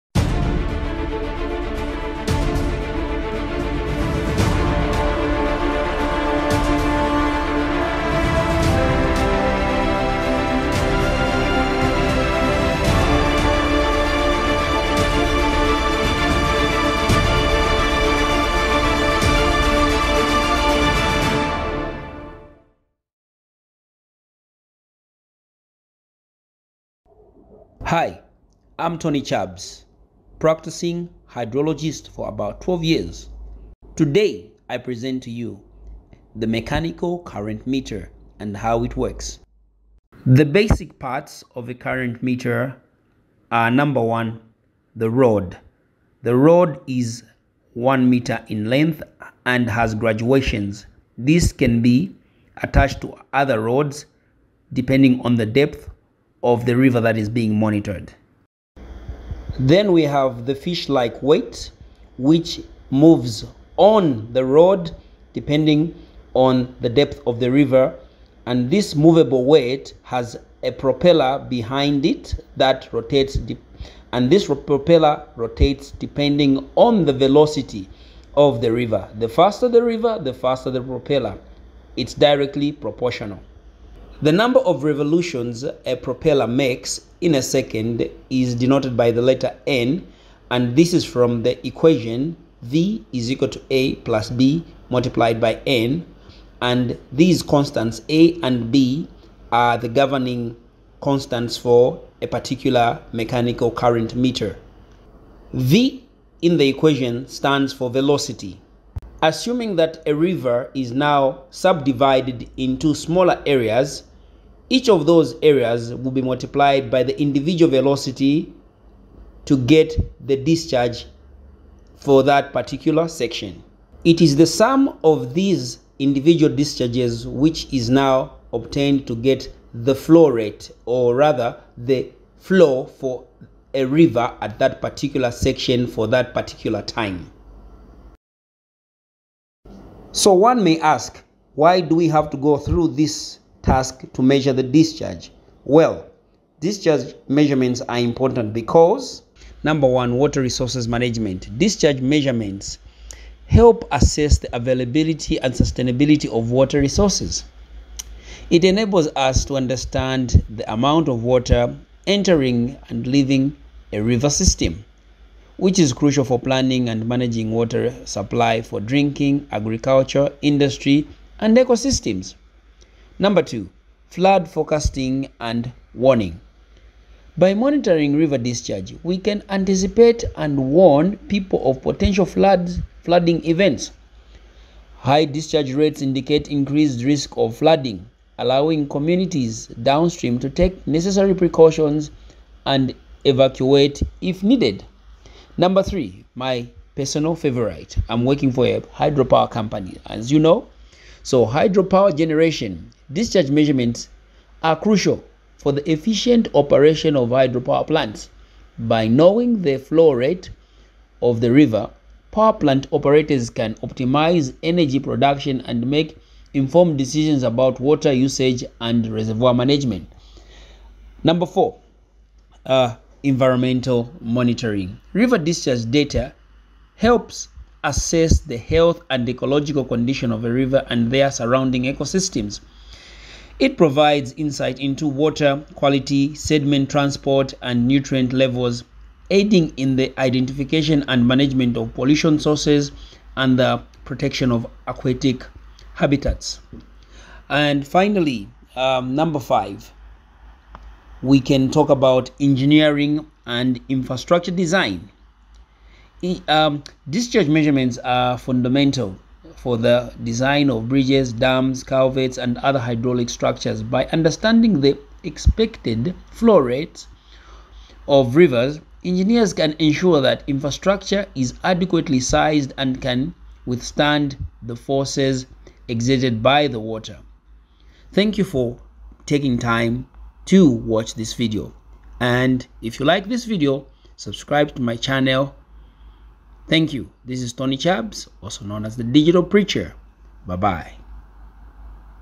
Hydrology - Flow measurements using a current meter | Eng.Tony Chabz
Explanation of the parts and use of a current meter flow discharge measurement